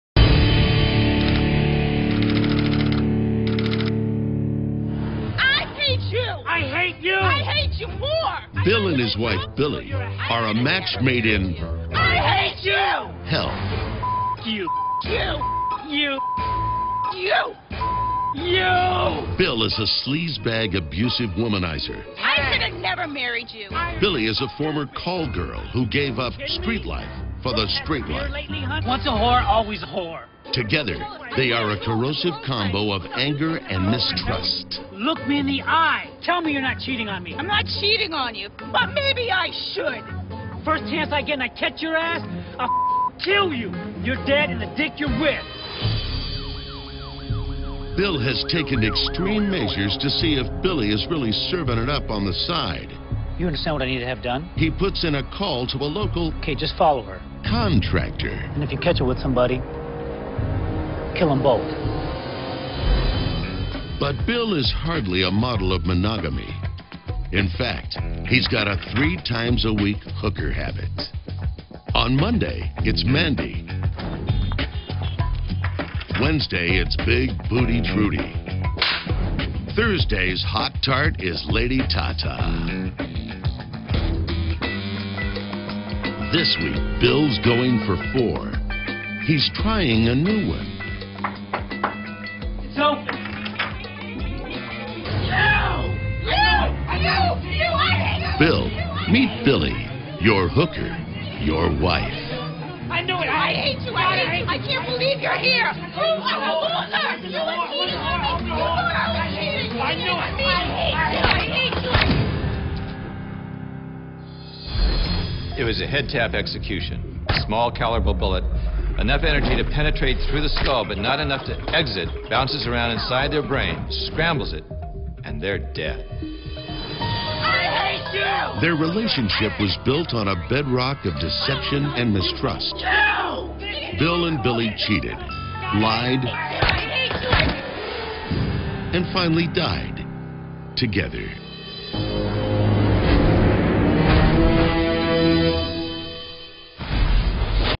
A FATAL ATTRACTION GONE WRONG
This married couple really cannot stand eachother. The trust in this relationship is gone, so the husband accuses his wife ( a former call-girl) of cheating,  she denies it, but tells him that she...